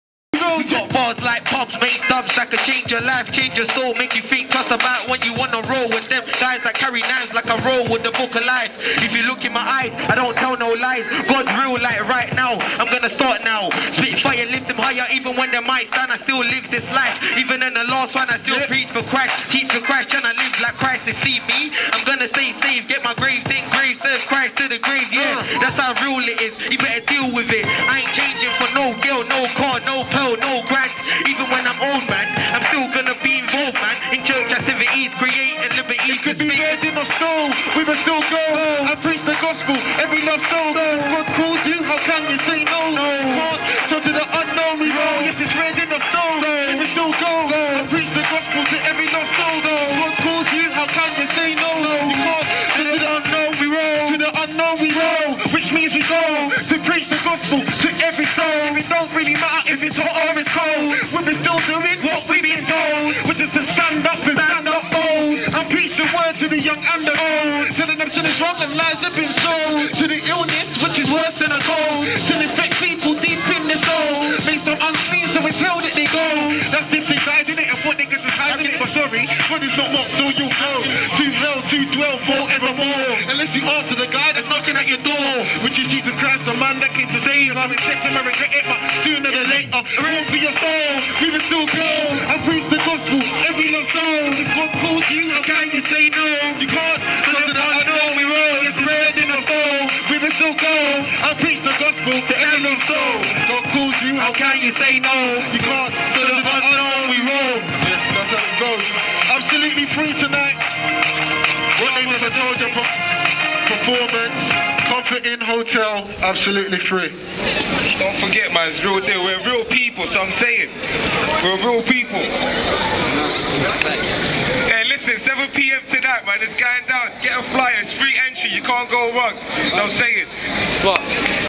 Reading boys
This was at Reading, U.K, they were promoting a venue they were gonna be playing at that night.